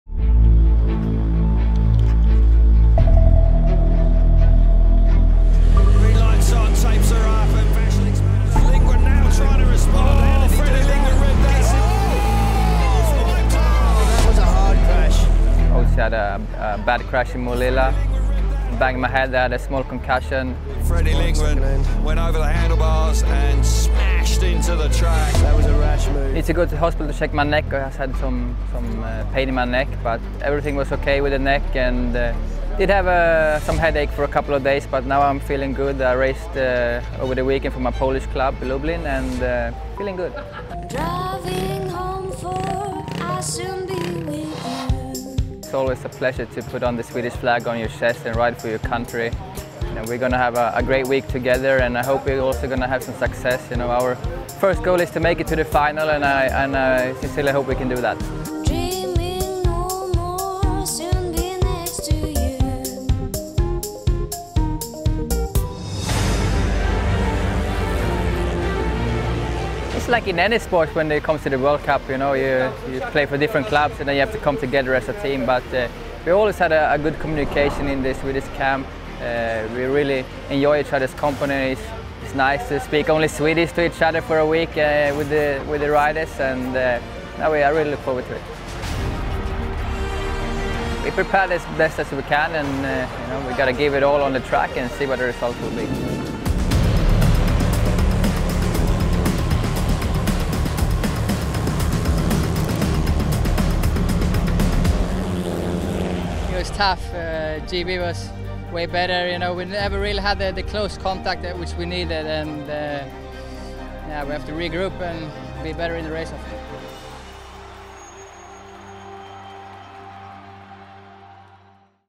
Lingren reflects on Semi-Final 1🎙️ SWC | FIM Speedway Grand Prix
From a minor concussion in Malilla to leading his country in the World Cup 💪

Fredrik Lindgren reflects on a tough fought result in SF1 👊